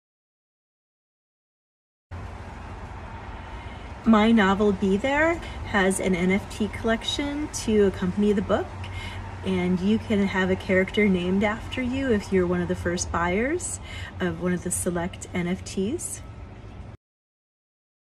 On Storytelling #173: NFT Book Marketing
Get a character named after you when you purchase a digital collectible and other book marketing tips to createeveryday virtually anywhere

For more on my Write Your Book online class, register